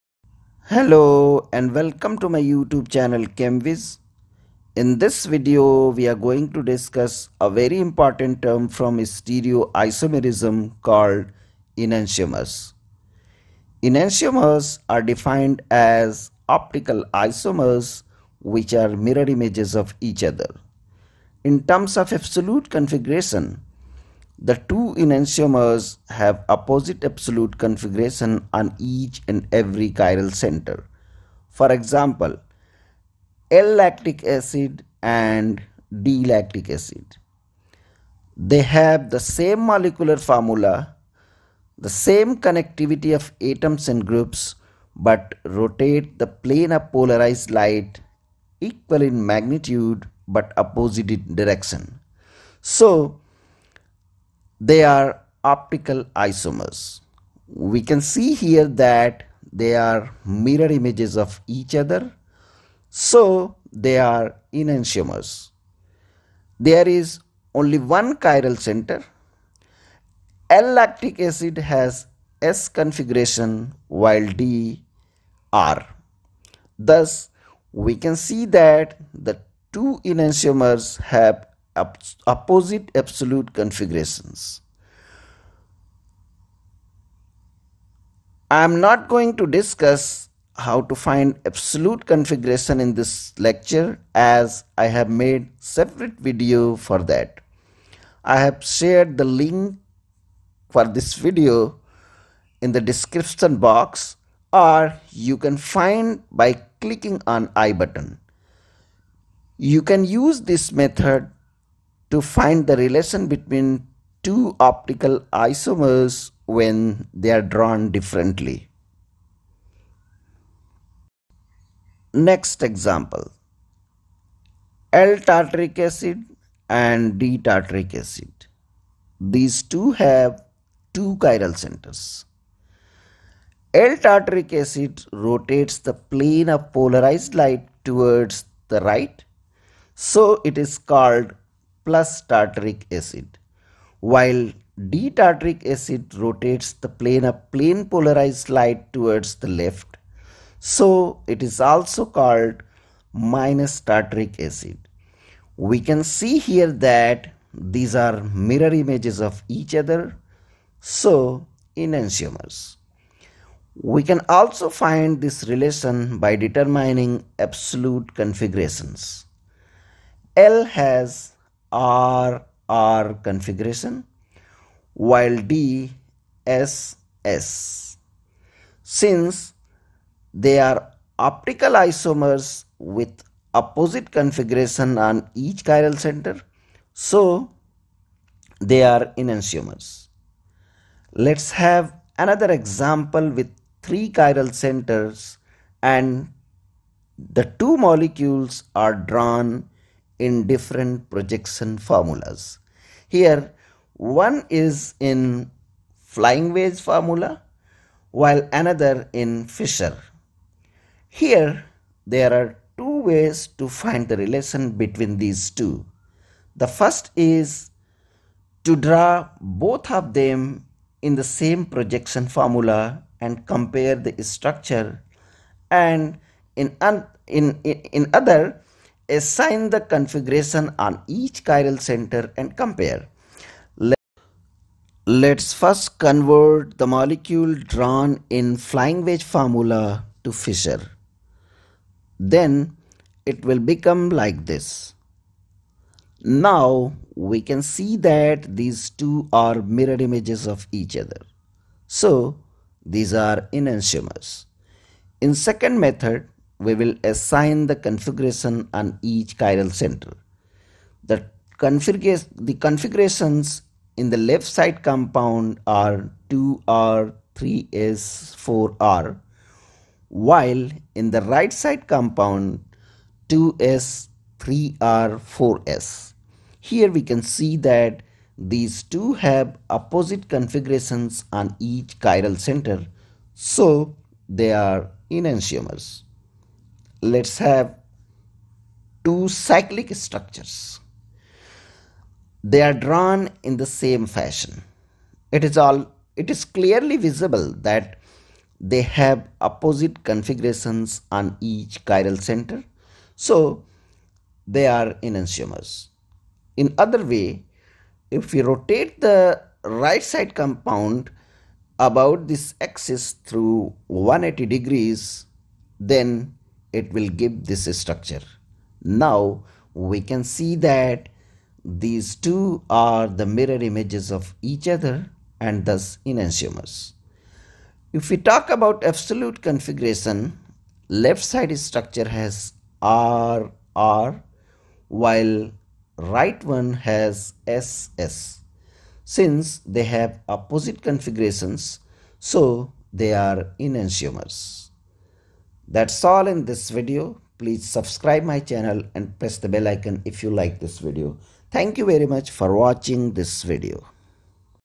Enantiomers
Enantiomers are optical isomers which are mirror images of each other. The two enantiomers have the same physical and chemical properties except their behavior towards the plane polarized light. One enantiomer rotates the plane of plan polarized right towards the left while other towards the right. In this video different examples of enantiomers along with the way to find the relation between them by their structures have been discussed in detail.
Optical isomerism
YouTube shorts